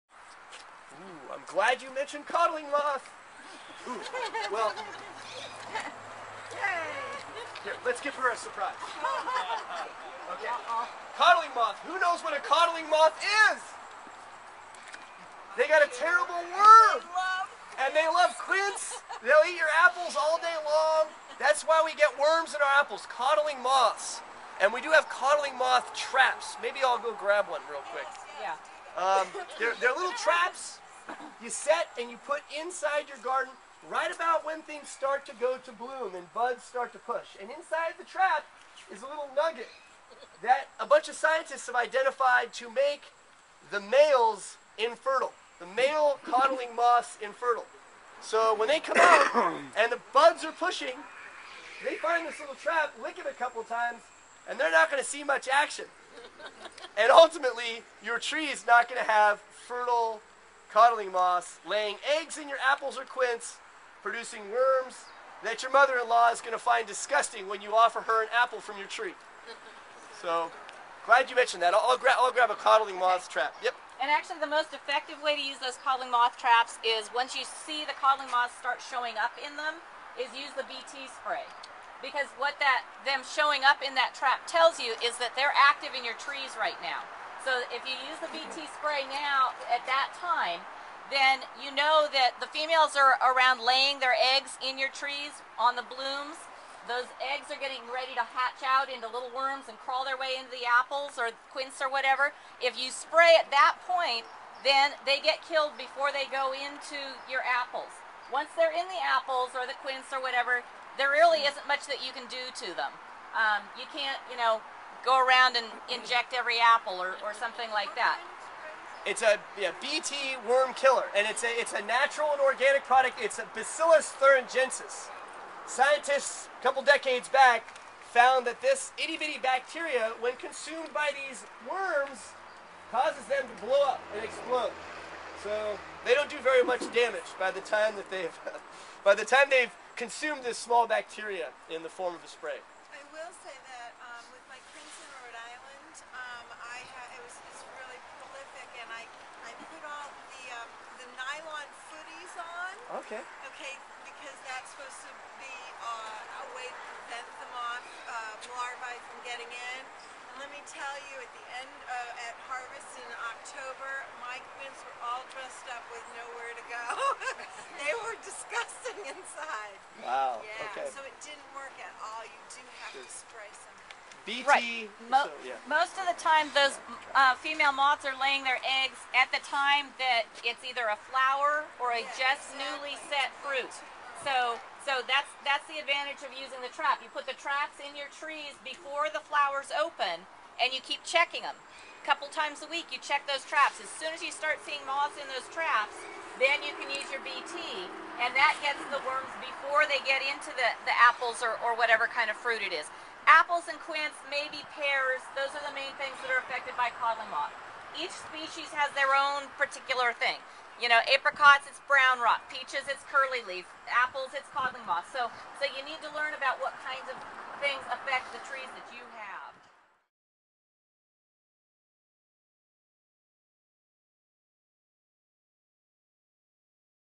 How To Protect Apple Trees From Codling Moth, Organic Options
Sign up for a newsletter and get a Free Leap Start Fertilizer Packet!

McShanes nursery has weekly workshops. In this video we learn about various methods of controlling Codling Moth in Apple Trees.

I am glad you mentioned codling moth! Well here lets give her a surprise. Who knows what a codling moth is? They got a terrible worm and love fruit trees and they eat your apples all day long. That's why we get worms in our apples, codling moths.

There little traps, you set and you put inside your garden right when things start to bloom and buds start to push and inside the trap is a little nugget that a bunch of scientists identified to make the males in feral. When they come out and the buds are pushing they find this trap, lick it a couple of times. After that the little guys are not going to see much action and your tree is not going to have fertile codling moths laying eggs in your apples or quince producing worms.
The most effective way to use those codling moth traps is once you see the codling moths start showing up in them. Use the Bacillus thuringiensis (or Bt) it's a naturally occurring bacteria. If you use the BT at that time then you know that the females are around laying their eggs in your trees on the blooms. Those eggs are getting ready to hatch out into little worms and crawl their way into the apples are quince or whatever. If you spray at that point (with BT) then they get killed before they go into your apples. Once they are in your fruit there really isn't much that you can do to them.

Scientist, a couple decades back found that this tiny bacteria when consumed by these worms causes them to blow up and explode so, they don't do very much damage by the time they have consumed this small bacteria in a form of a spray.
Most of the time those female are laying there eggs at the time that its either a flower or just newly set fruit. So, that the advantage of using the traps, you put the traps in your trees before the flowers open and keep checking them a couple times a week. You check those traps as soon as you start seeing moths in those traps then you can use your BT and that gets the worms before you get into the apples or whatever kind of fruit it is. Apple and quince and maybe pears those are the main things that are effected by codling moths. Each species has there own particular thing. Apricots its brown rot. Peach's its curly leaf. So, you need to learn what kinds of things effect the trees that you have.